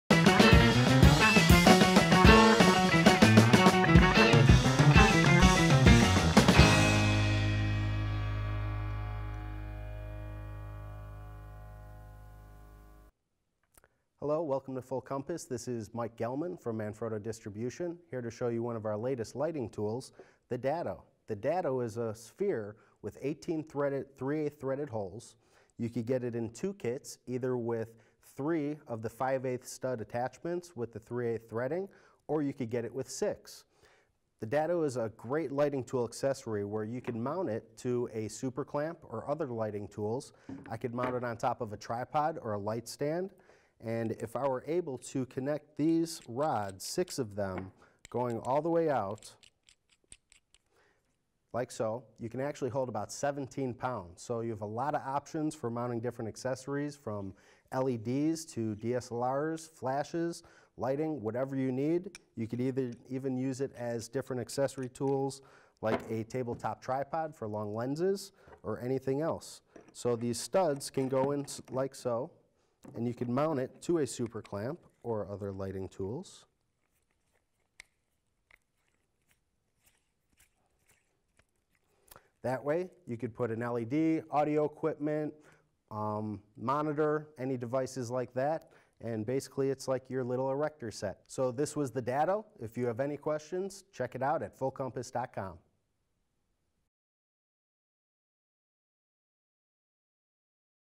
Manfrotto DADO Universal Mounting for Cameras and Lighting Accessories Overview | Full Compass
Manfrotto's Mike Gelman demonstrates the Manfrotto DADO Universal Mounting Solution at Full Compass Systems.

The Manfrotto DADO is a universal camera and lighting mounting solution for Videographers and Photographers. The universal threading fits most mounts and accessories.